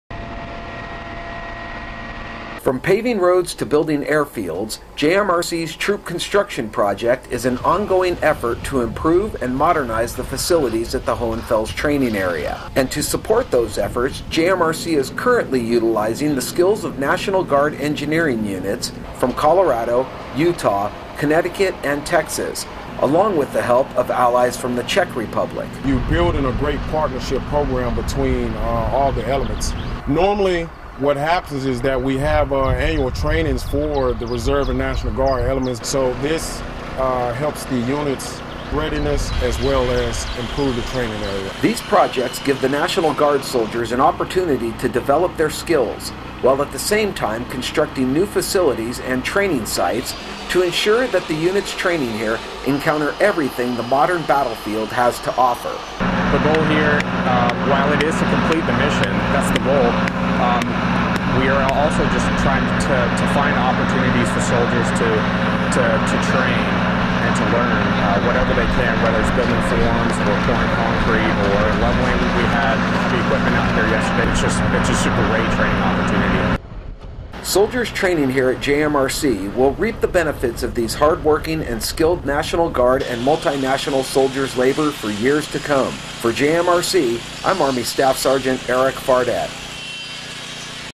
Guard, Active, and Reserve Working Together in Germany
All three components of the Army are working together to complete a major objective, combat readiness. The Joint Multinational Readiness Center (JMRC) also included allies from the Czech Republic, which enhanced global partnerships and international relations while the training took place in Hohenfels, Germany. This engineering objective enhanced the total Army partnership while working in an overseas environment and The JMRC was essential in improving combat readiness across the force by updating the training facilities in Hohenfels, providing a more realistic training environment that better matches today's combat requirements for readiness.

Video by Staff Sgt. Erik Fardette